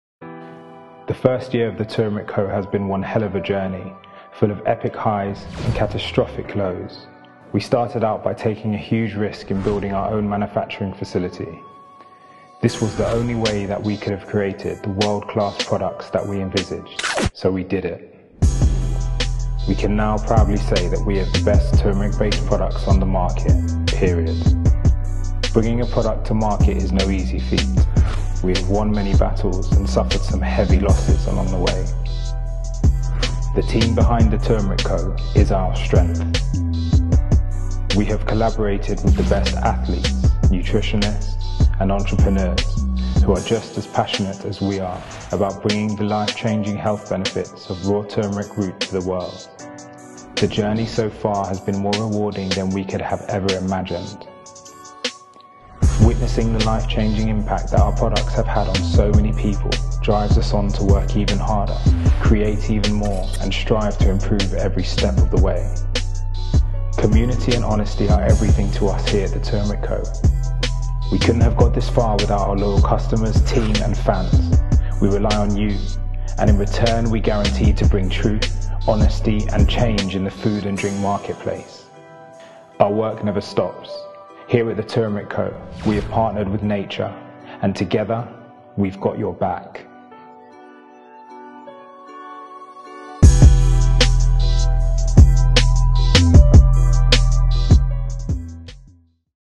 The Turmeric Co. Story So Far
Our founder Thomas Roson-Kanu speaks about the passion behind the Turmeric Co. as we are taken on a snapshot journey to what they company has already achieved and what it will achieve in the future.